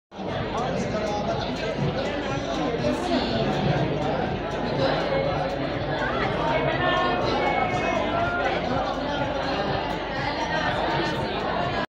ZGLAMTV : LIVE ACARA KARPET MERAH ASK TV3 2019
Kemeriahan bermula bila acara Karpet Merah
dibuka.
 Anugerah Skrin TV3 2019
PWTC
1 November 2019